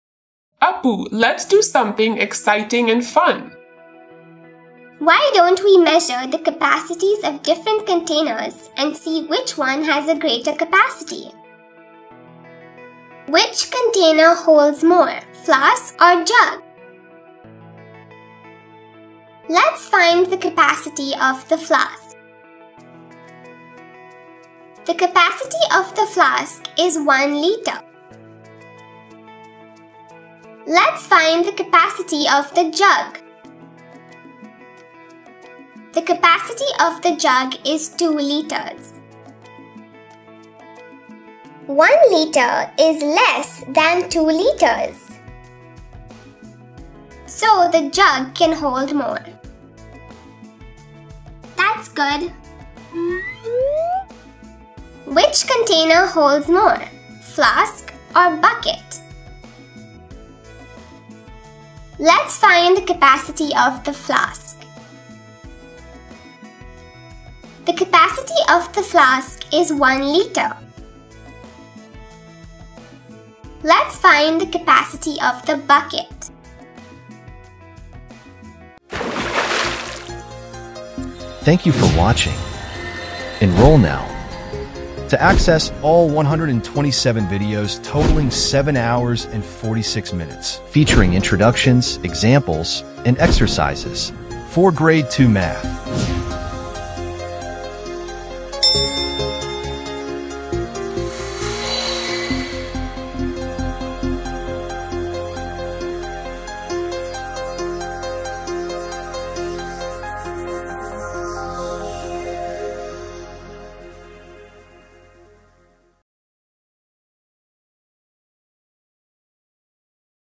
G2 - Module 21 - Exercise 2 - Which container holds more? | Appu Series | Grade 2 Math Learning
Which container holds more? Let’s find out with Appu and Tiger! In this exciting challenge, you’ll compare different containers and figure out which one has the most capacity. It’s a fun way to sharpen your math skills!